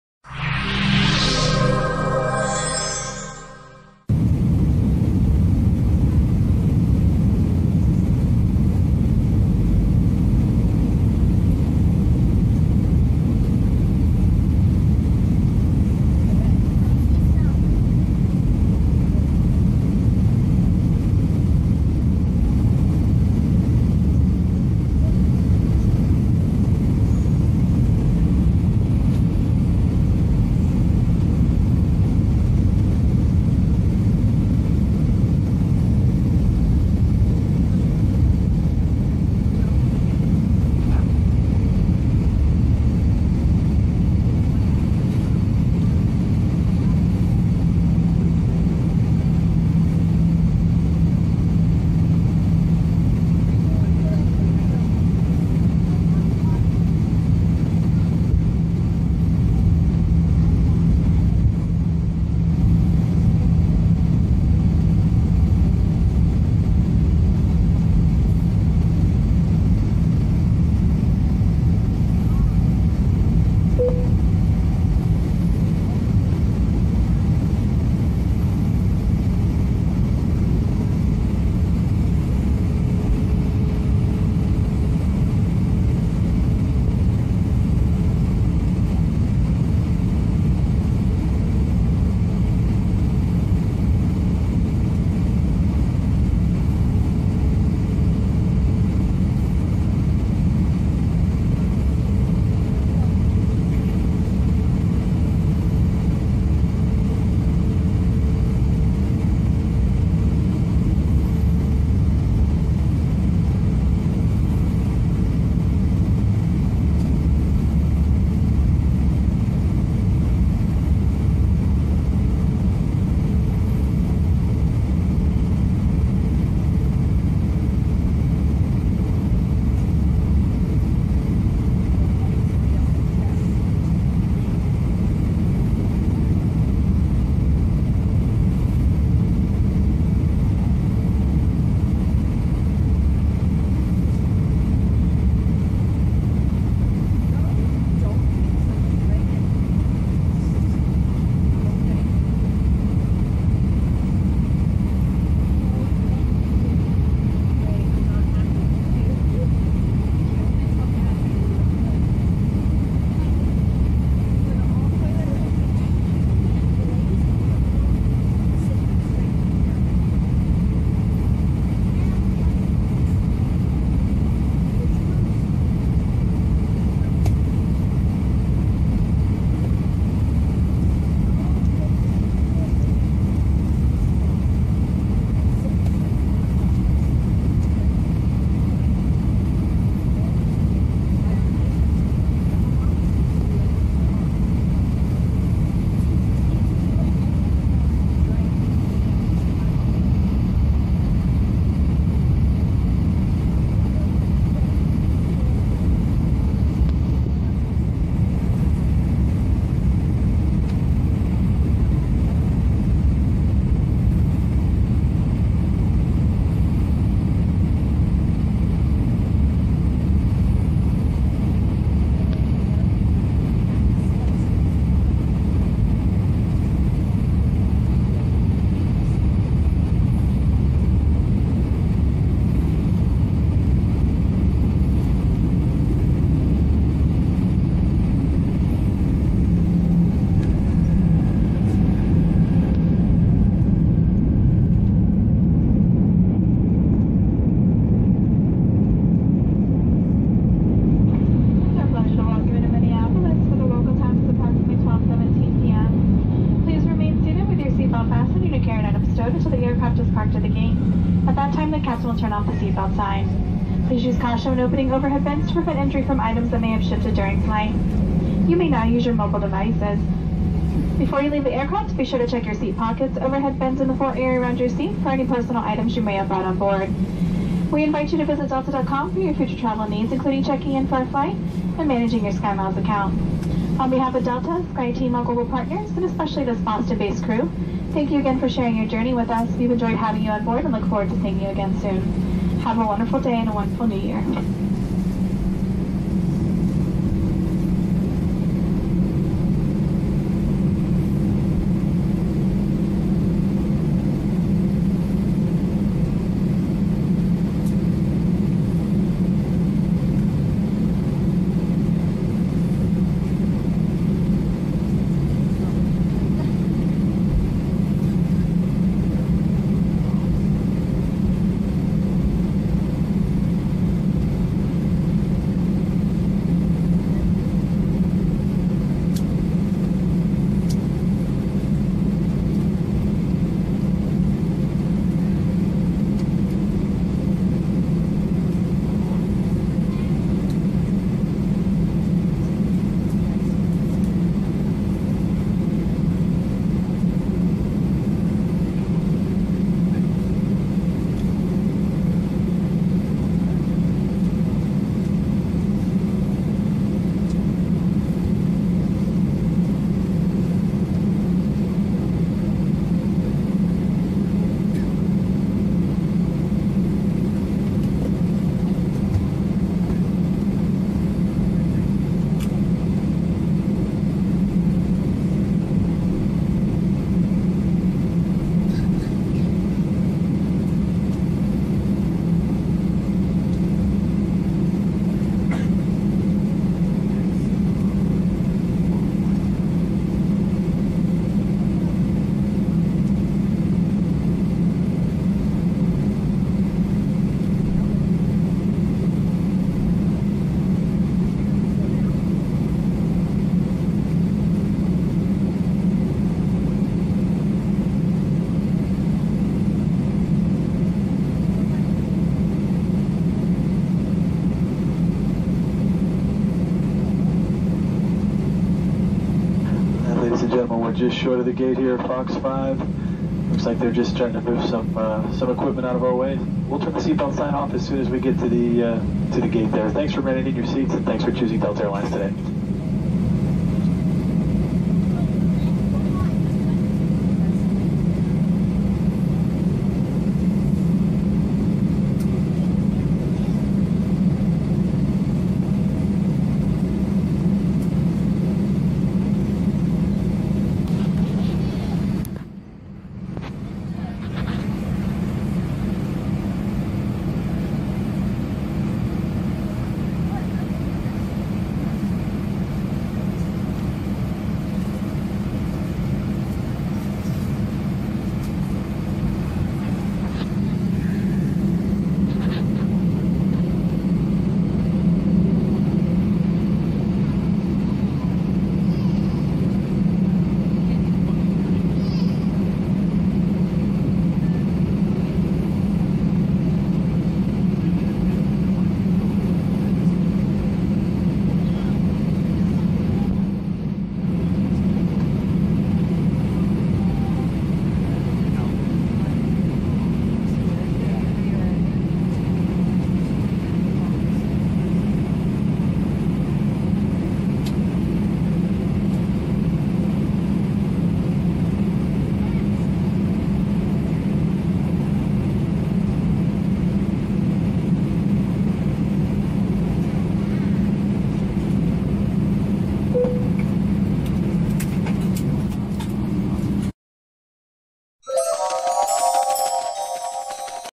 VIRTUAL REALITY AIRLINER LANDING IN MINNEAPOLIS (FREE)
Winter landing window seat of a Delta Airlines flight.  Excellent virtual reality experience for your smartphone!   Cool to watch just by itself.   Approach over highway 52 from the south.